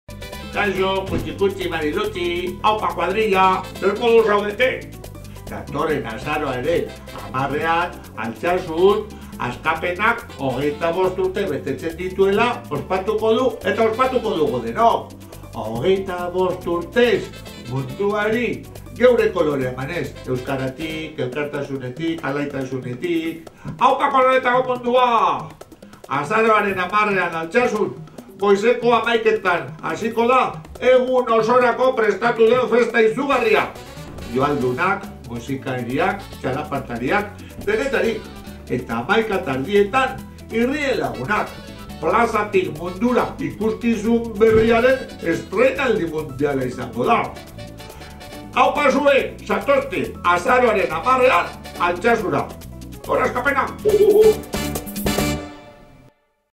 Porrotx, irrien lagunak, 'Askapenak 25 urte'
Porrotxek dei egiten du Azaroaren 10ean Altsasura joateko, Askapenaren 25. urteurrena ospatzeko, bertan irrien lagunak-en emanaldi berria estrenatuko da gainera: 'Plazatik mundura' eta denok batera ormairudi bat margotuko dugu